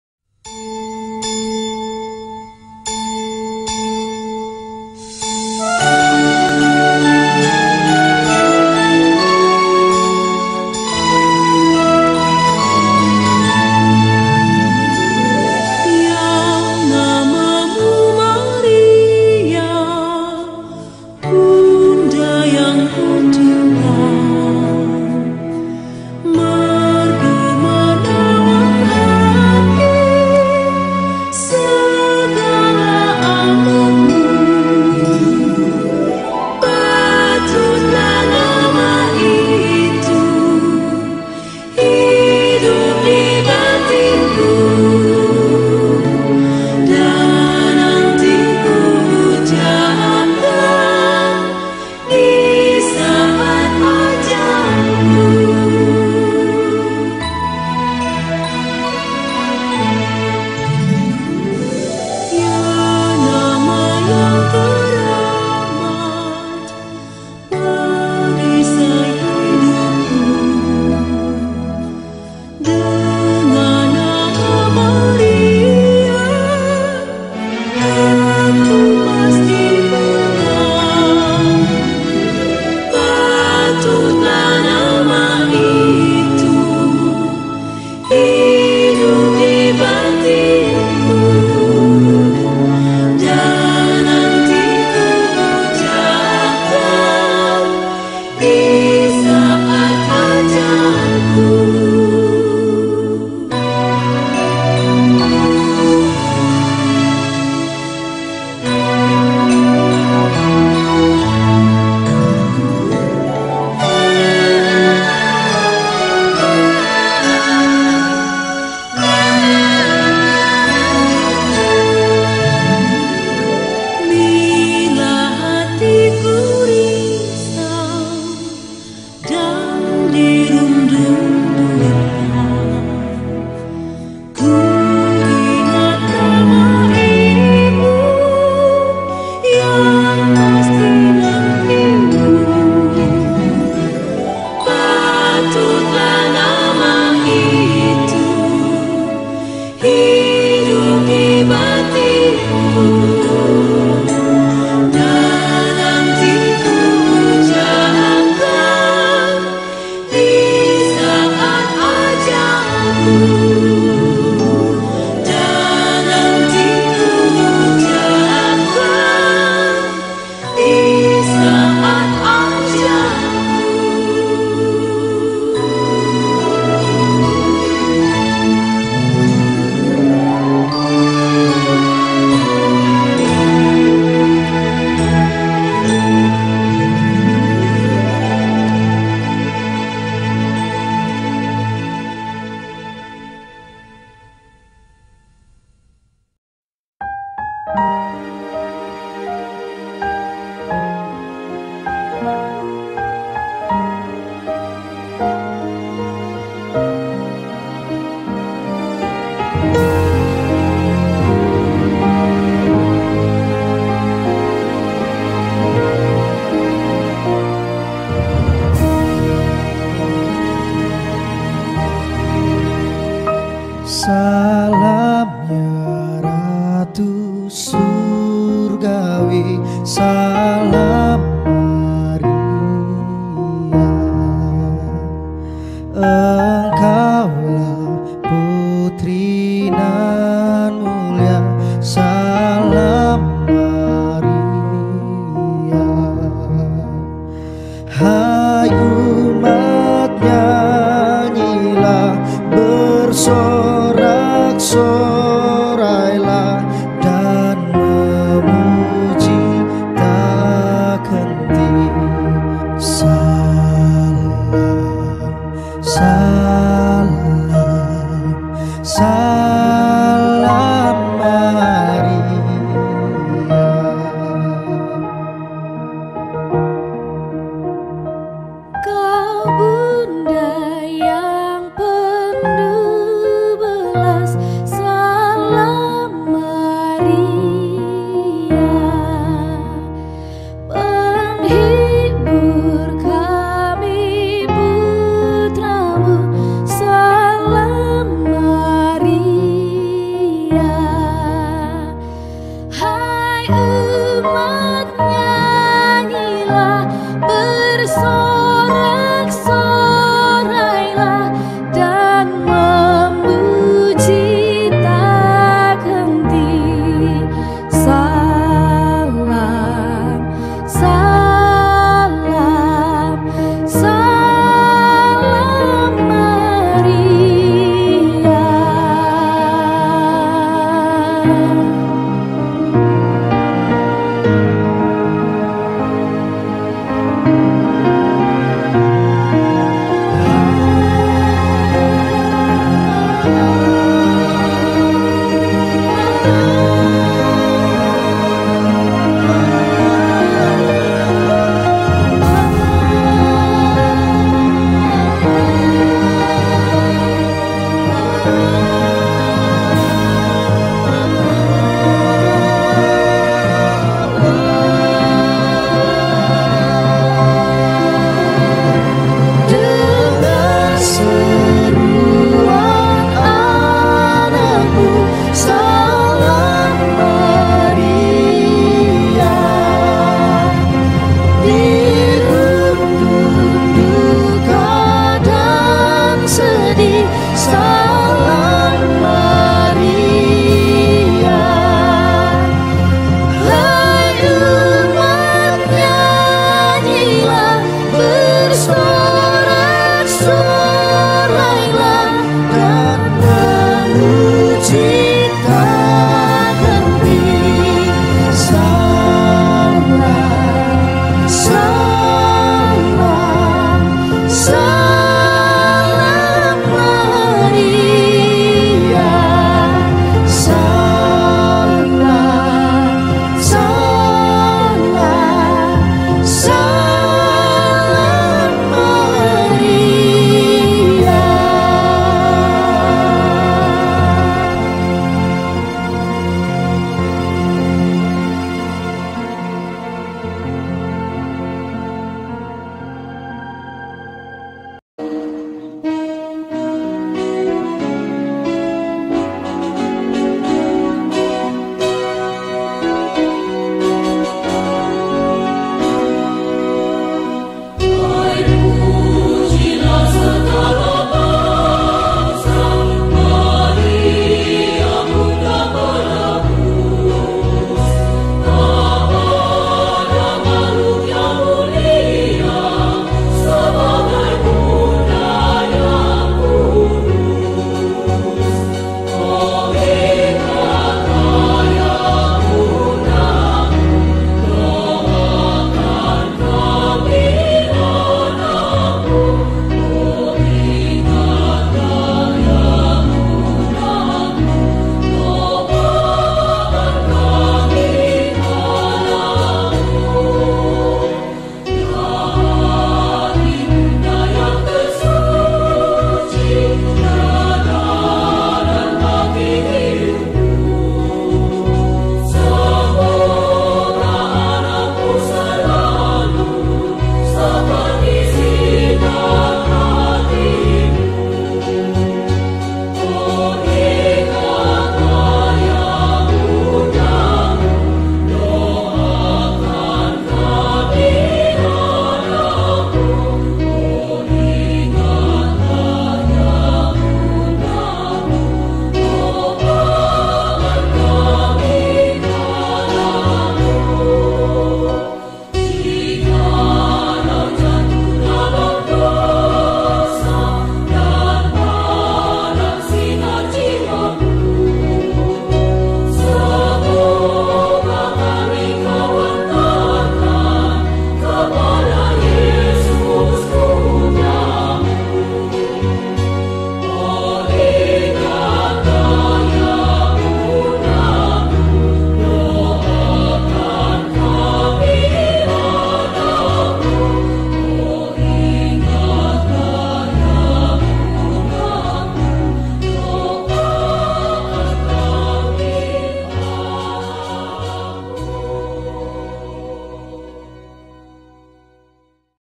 3 LAGU BUNDA MARIA with lyrics - Chatolica Nusantara
Selamat hari minggu untuk semua saudara-saudara terkasih, pada penutupan rosario ini channel kami CHATOLICA NUSANTARA mengunggah video yang berisi tiga lagu ibadat rosario yang sering dinyanyikan umat katolik pada bulan rosario ini, semoga pada tanggal 31 Mei 2020 ini, tepatnya pada penutupan rosario ini, semua orang dapat berdoa kepada Bunda Maria, Bunda sang Juru selamat kita Yesus Kristus, agar dapat selalu Bunda Maria mendoakan kita agar diberikan kesehatan, kebahagian, serta kelancaran rejeki selalu, Jesus Bless You, Amen.